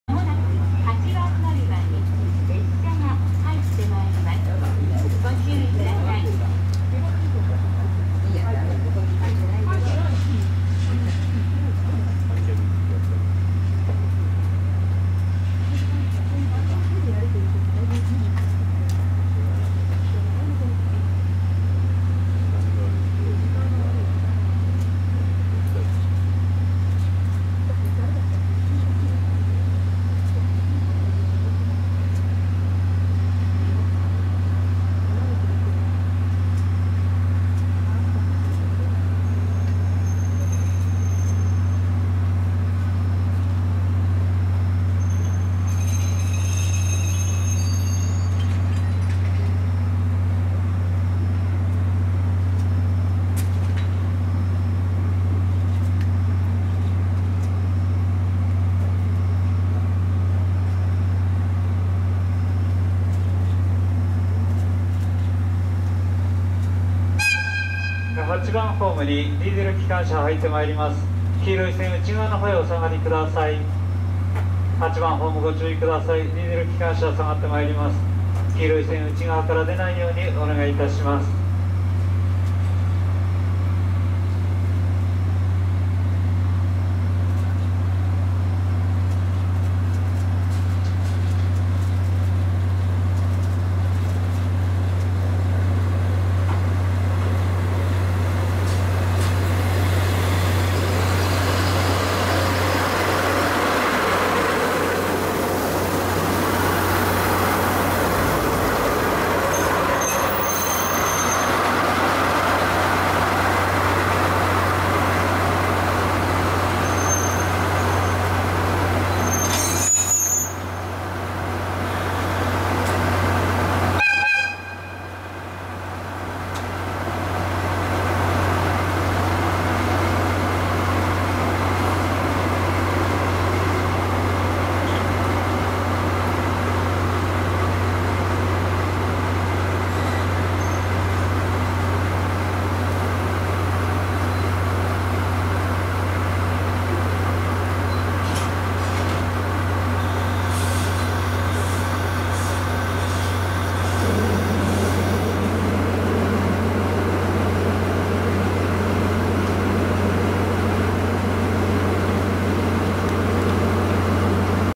函館駅 急行はまなす DD51に機関車付け替え
この時のはまなすは編成が長かったのに１両で引っ張るんだな　　と思いました。
北斗星とかも同じような長さで２両でひっぱってるのにな